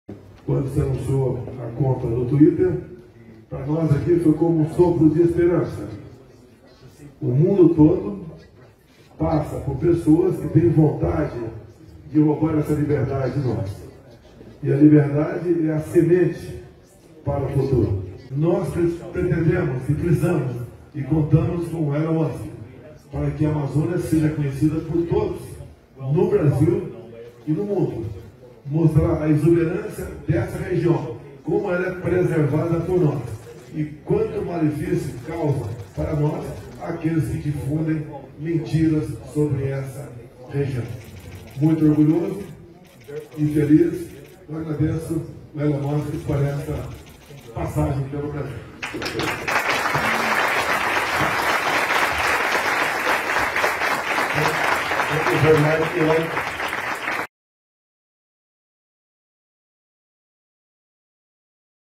Bolsonaro recibe a Elon Musk en Brasil: "Usted es un protector de la libertad"
El hombre más rico del mundo, Elon Musk, sigue dando de qué hablar. Esta vez, tras una invitación formal del ministro de Comunicación de Brasil, Fábio Faria, decidió viajar a Porto Feliz, en el interior del Estado de São Paulo, para presentar su inversión en Starlink Brasil.

REDES DE LA DERECHA DIARIO.